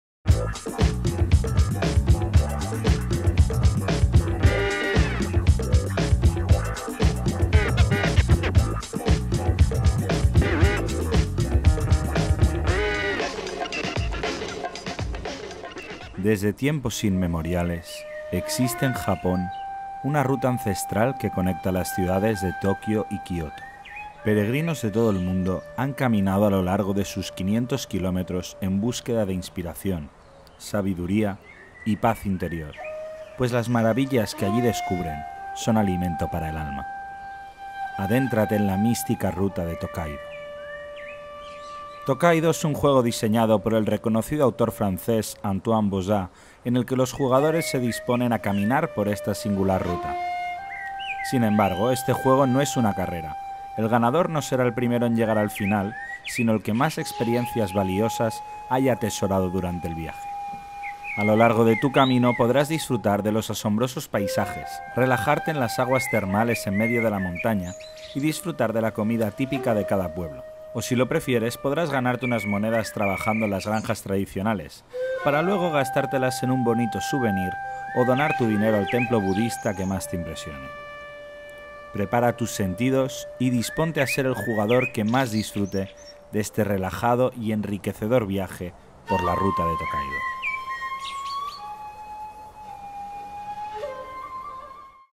Tokaido - Tráiler
Recorre la Tokaido, la famosa ruta entre Tokyo y Kyoto. Atraviesa parajes magníficos, saborea deliciosas especialidades culinarias, compra souvenirs y no te olvides de visitar los baños termales.